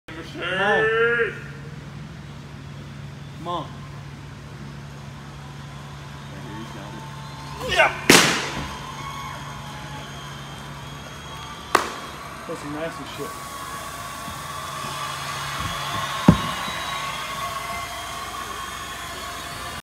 David Aardsma 90mph at 40 years old
Learn how mobility affects your ability to move efficiently to look smooth on the baseball field.